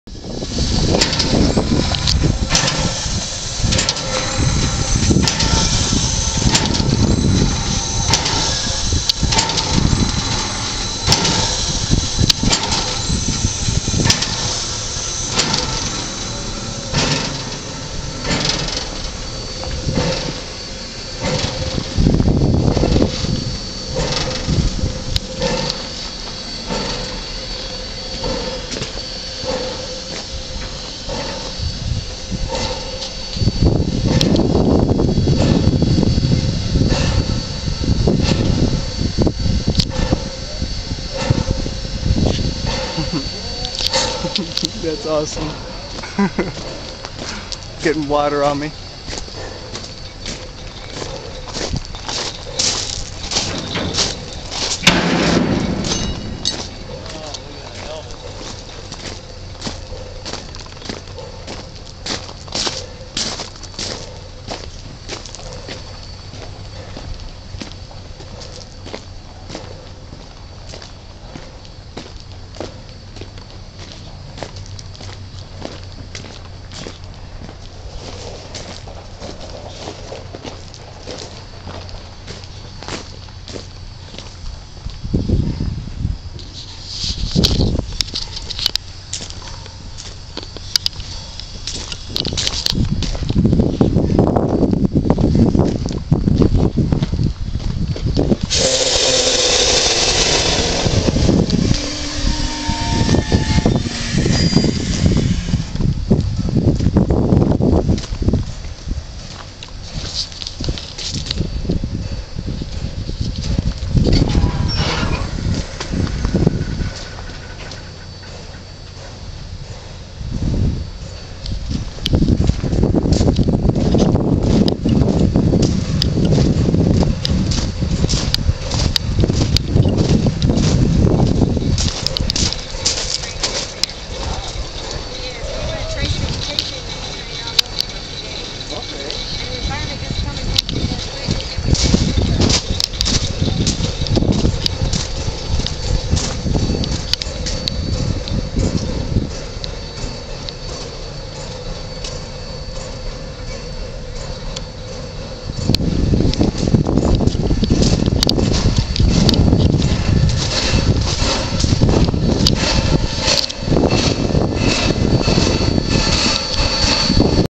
A look over of 765
Nickle Plate 765 rest at Homestead Yard in Toledo,Ohio. At 7:00am the next morning, she'll depart Homestead and go to Bellevue, Ohio.